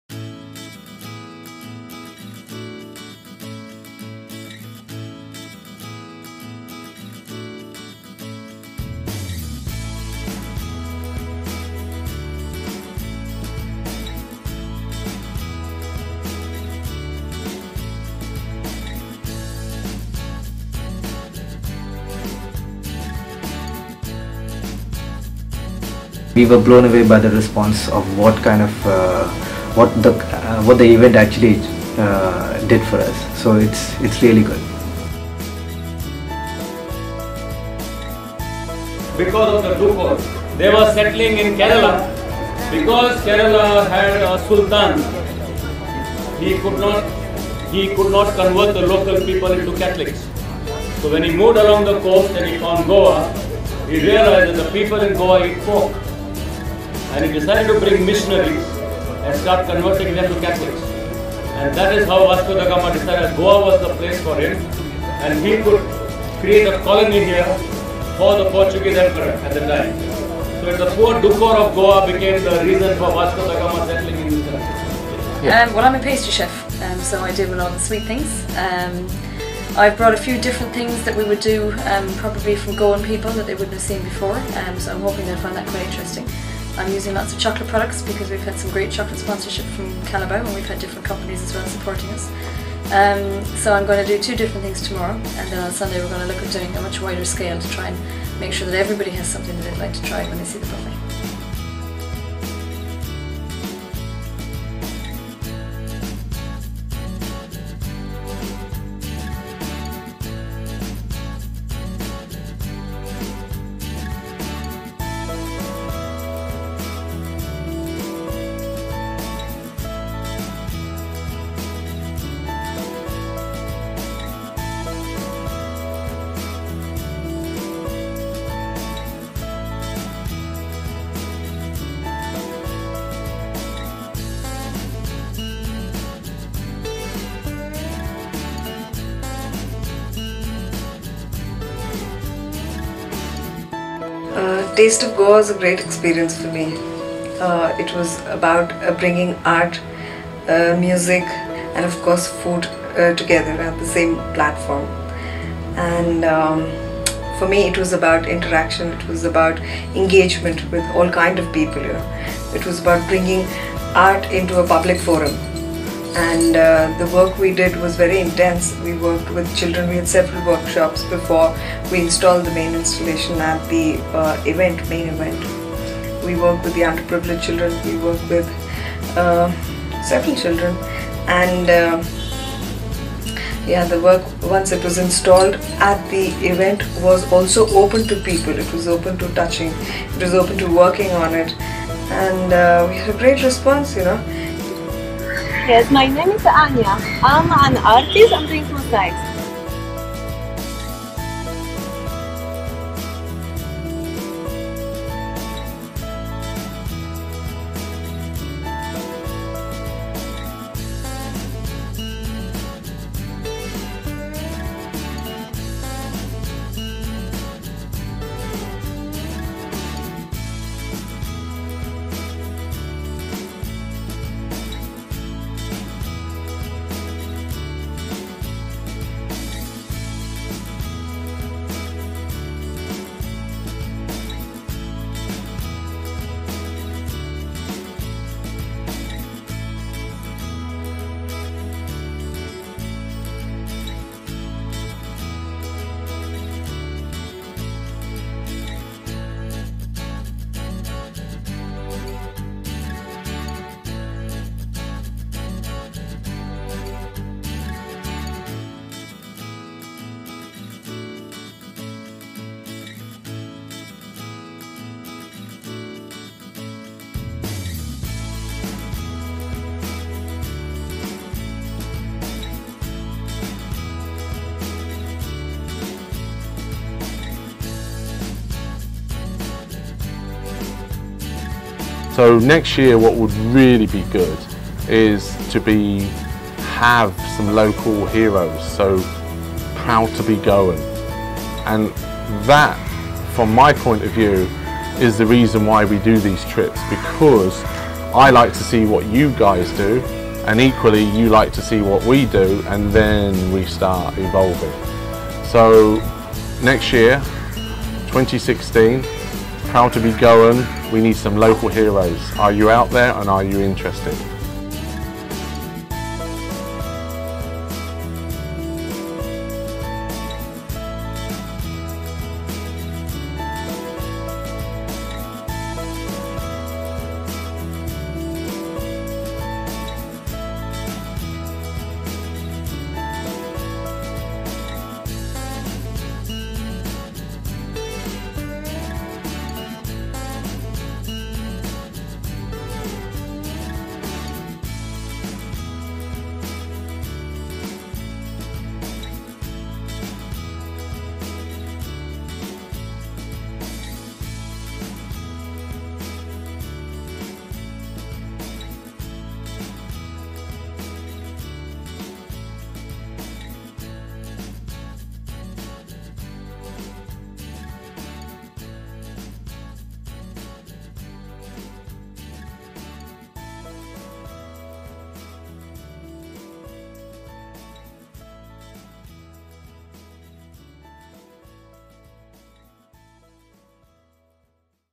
Taste of Goa Festival
Taste of Goa is our brand which promotes food, music, art, heritage, culture and other subtler aspects of Goa.

We organised our first festival at the Garcia De Orta Garden in Panjim. We had five British Chefs who joined us from London including Chef Cyrus Todiwala OBE (Order of The British Empire).

Music, Art, Food,  Heritage etc were a part of the festival. We ad stalls by Goa's most renowned restaurants, housewives, art centers etc. National and International bands enthralled the crowd.